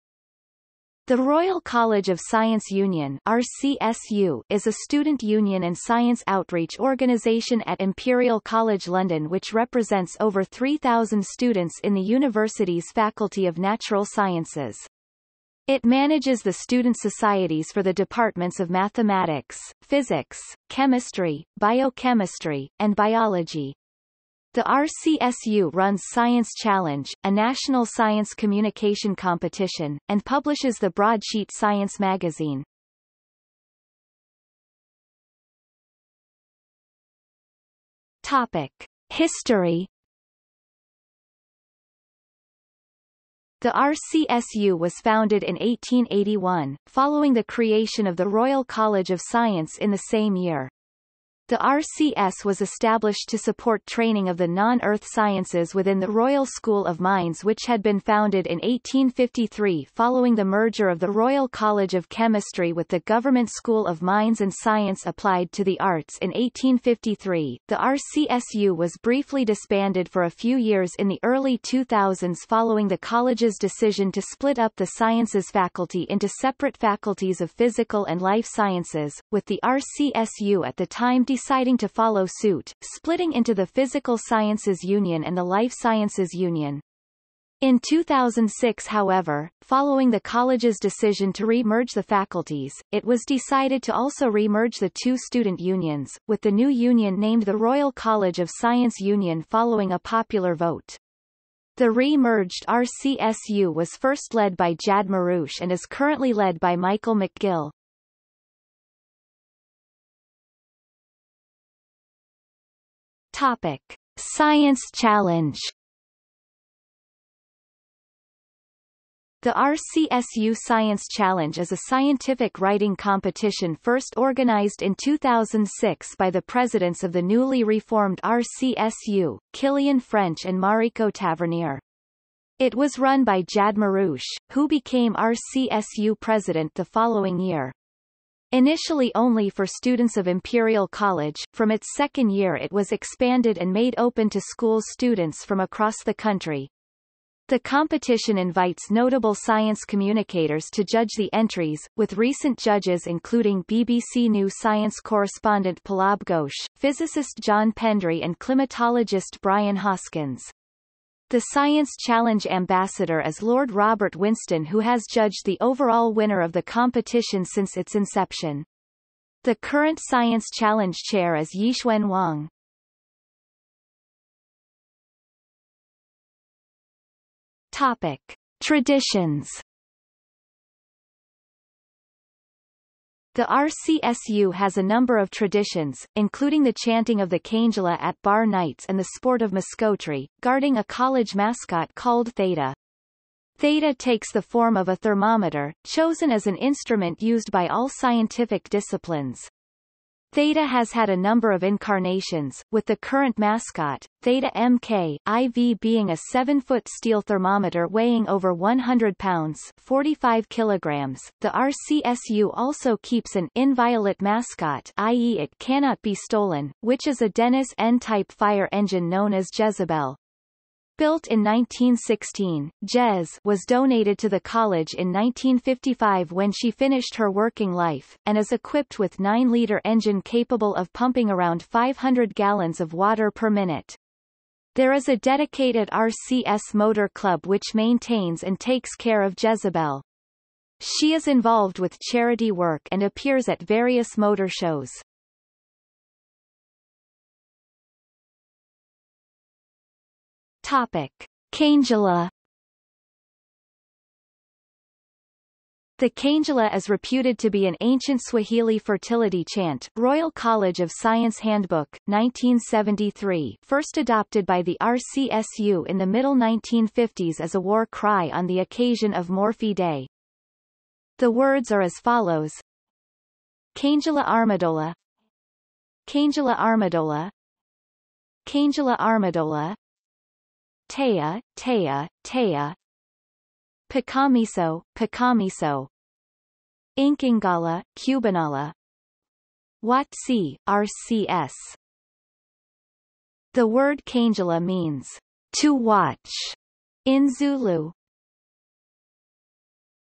Royal College of Science Union | Wikipedia audio article
00:00:34 1 History
00:01:49 2 Science Challenge
00:02:54 3 Traditions
00:04:07 4 Kangela

- Socrates

SUMMARY
The Royal College of Science Union (RCSU) is a student union and science outreach organisation at Imperial College London which represents over 3,000 students in the university's Faculty of Natural Sciences. It manages the student societies for the departments of Mathematics, Physics, Chemistry, Biochemistry, and Biology. The RCSU runs Science Challenge, a national science communication competition, and publishes the Broadsheet science magazine.